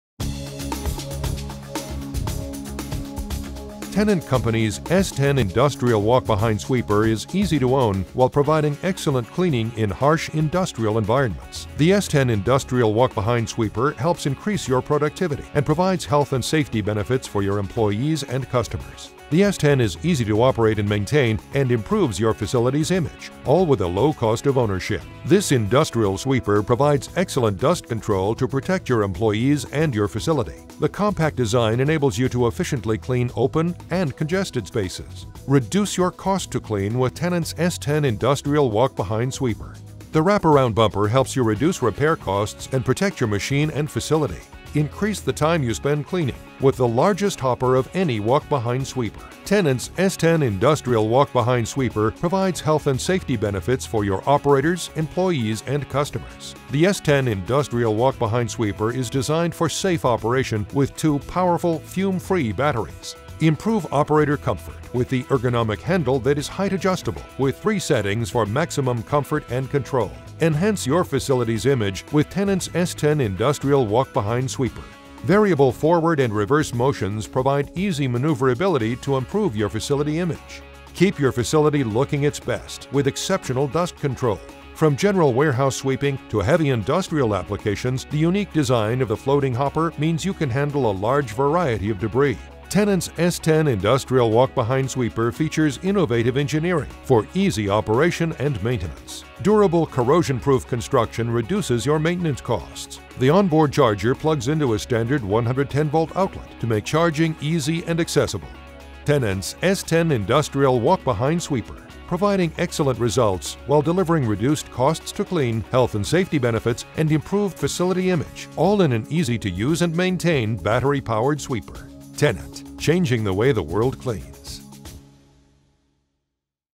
Tennant S10 Walk Behind Sweeper
The Tennant S10 walk behind sweeper is a heavy duty industrial battery powered machine for work in harsh environments. On-board dust suppression captures dust particles down to 3 microns and filter cleaning maintains performance.